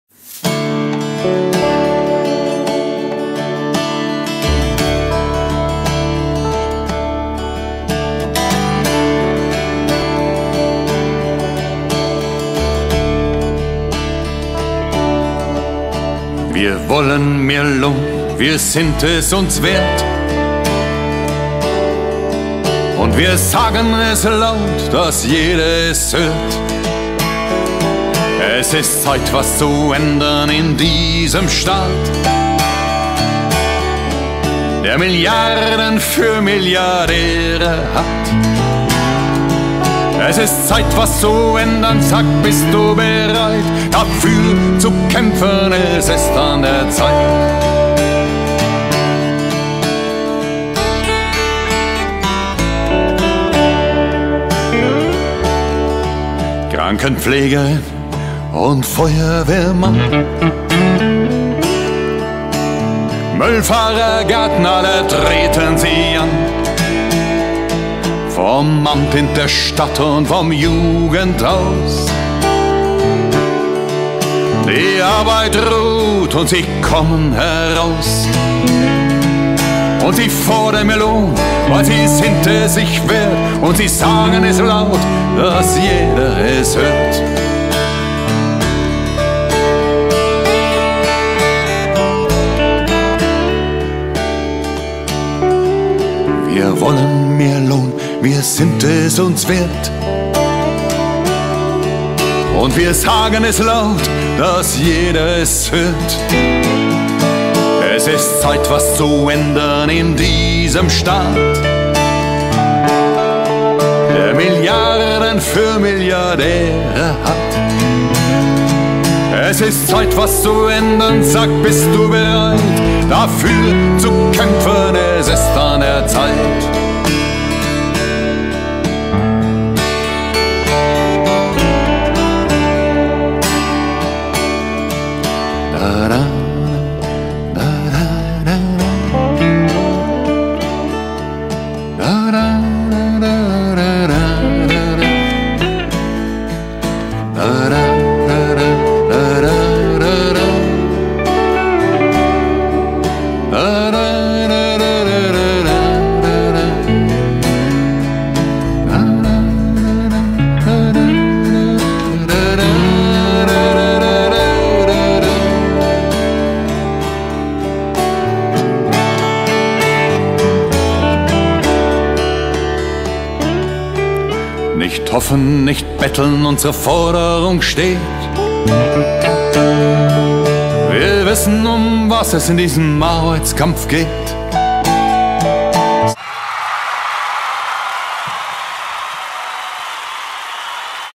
Warnstreik 2
Der zweite Warnstreik am UK Aachen am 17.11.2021